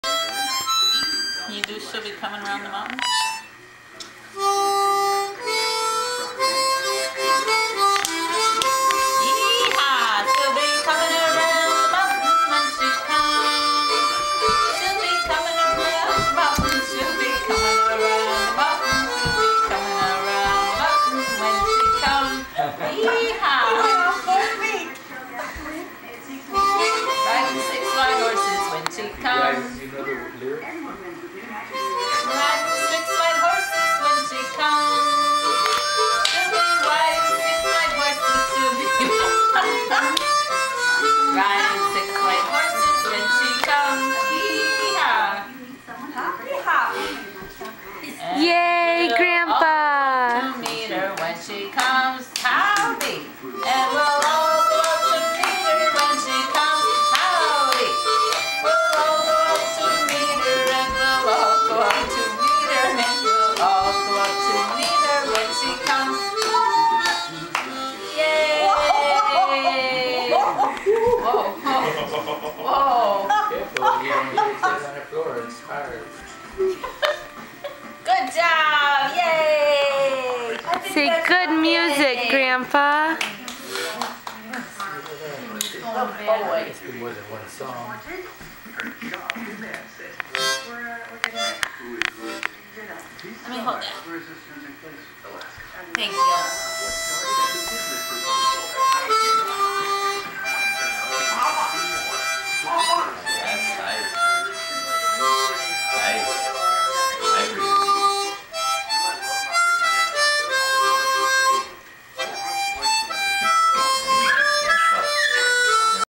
Brady and Grandpa
Brady dances as Papa Bob plays harmonica.